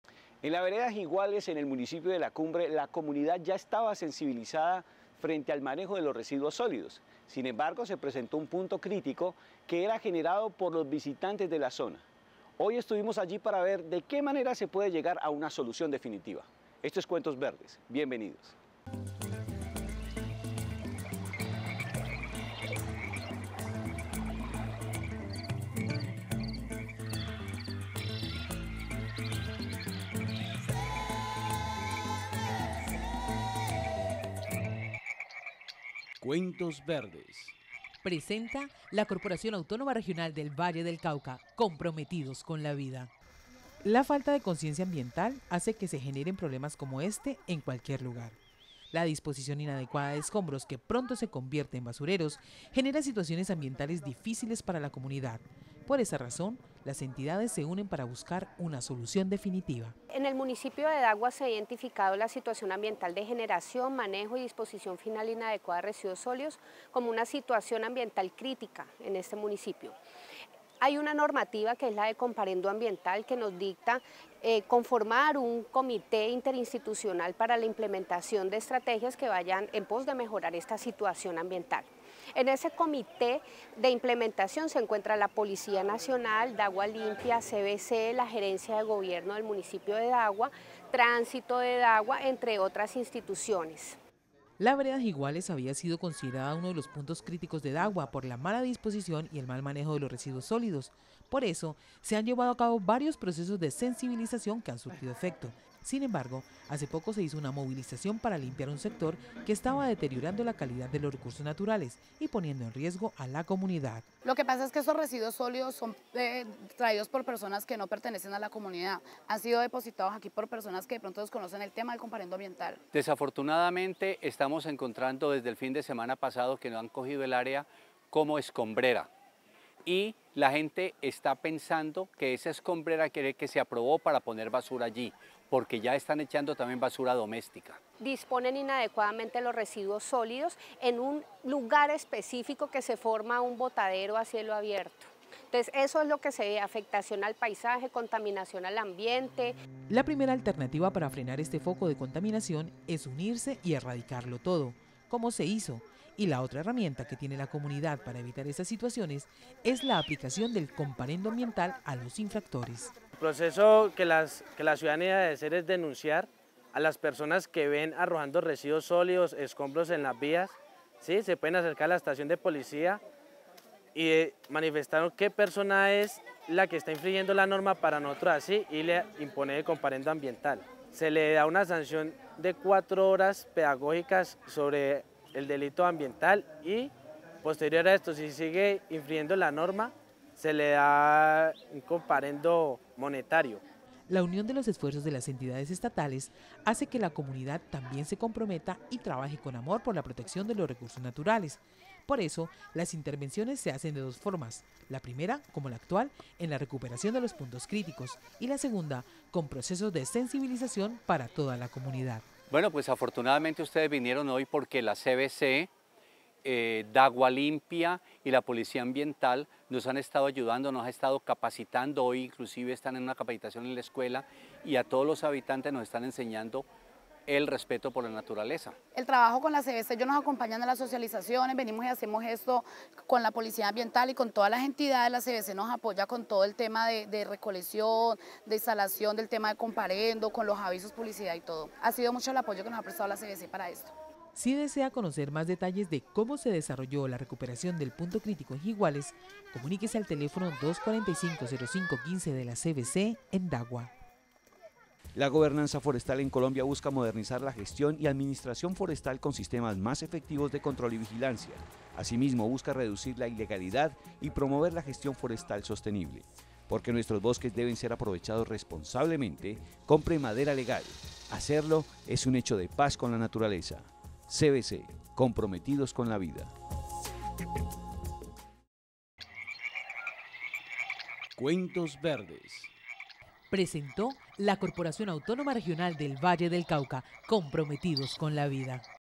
Sensibilización Punto Crítico Jiguales - Cuentos Verdes 16 de diciembre de 2016
SENSIBILIZACIÓN PUNTO CRÍTICO JIGUALES
En la vereda Jiguales de La Cumbre la comunidad ya ha sido sensibilizada frente al manejo de los residuos sólidos sin embargo se ha presentado un punto crítico por el mal manejo de los residuos sólidos que son generados por los visitantes de la zona, hoy veremos que se está haciendo para buscar una solución definitiva
Más información: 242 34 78 B/tura – Profesional. Paola Arango  / Com. Soc. Karol Arango